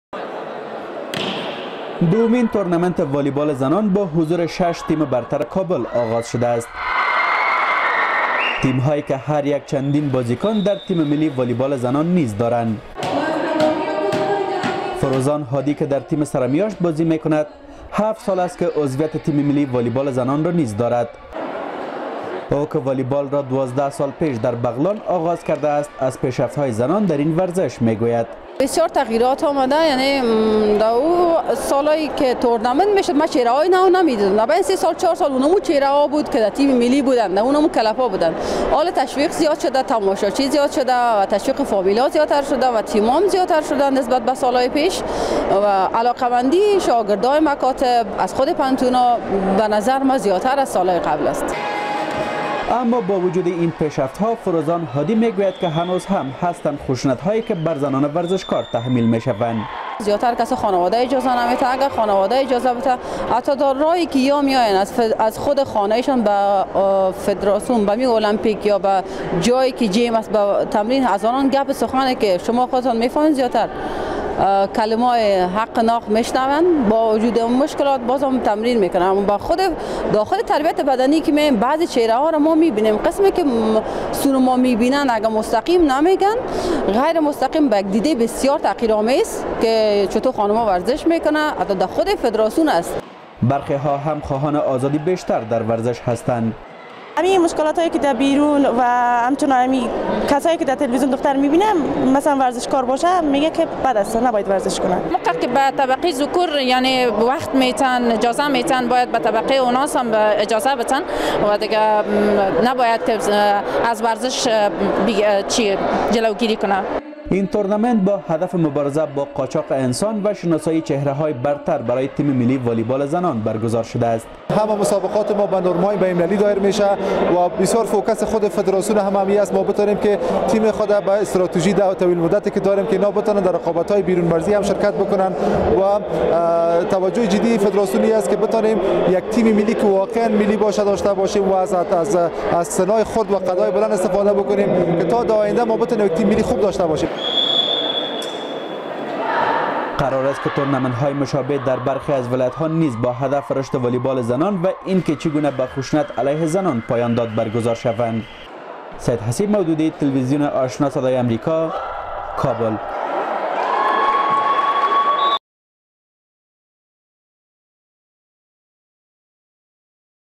Afghanistan girls volleyball - VOA TV Ashna
همزمان با آغاز دومین تورنمنت والیبال زنان در کابل، برخی از اعضای این تیم دختران، از تجارب خود در پیوستن به ورزش به صدای امریکا حکایت کردند، فروزان هادی که عضو تیم ملی والیبال زنان افغانستان است، می گـوید با وجودیکه والیبال زنان پیشرفت کرده است، اما هنوزهم آن ها با انواع خشونت و نگاه های تحقیرآمیز مواجه اند.